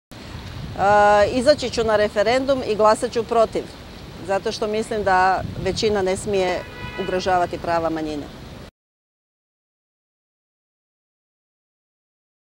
Ana Pezelj - GLASAM PROTIV!
˝Izaći ću na referendum i glasat ću protiv. Zato što mislim da većina ne smije ugrožavati prava manjina.˝ =